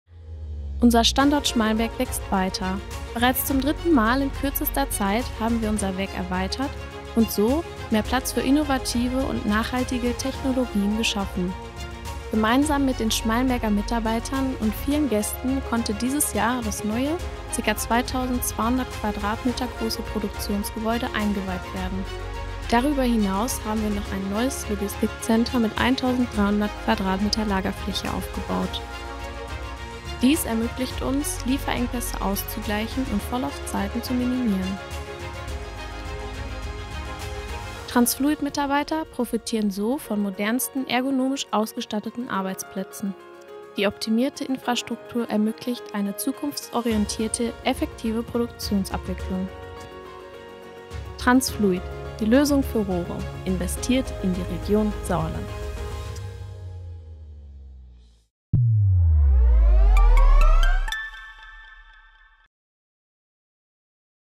transfluid GmbH [ Unser Standort Schmallenberg wächst weiter. ]
Bereits zum dritten Mal in kürzester Zeit haben wir unser Werk erweitert und so mehr Platz für innovative und nachhaltige Technologien geschaffen.

Gemeinsam mit den Schmallenberger Mitarbeitern und vielen Gästen konnte dieses Jahr das neue, ca. 2200 qm große Produktionsgebäude eingeweiht werden.

Darüber hinaus haben wir noch ein neues Logistikcenter mit 1300 qm Lagerfläche aufgebaut. Dies ermöglicht uns Lieferengpässe auszugleichen und Vorlaufzeiten zu minimieren.

Transfluid Mitarbeiter profitieren so von modernsten ergonomisch ausgestatteten Arbeitsplätzen.

Die optimierte Infrastruktur ermöglicht eine zukunftsorientierte effektive Produktionsabwicklung.

transfluid: Die Lösung für Rohre. Investiert in die Region Sauerland.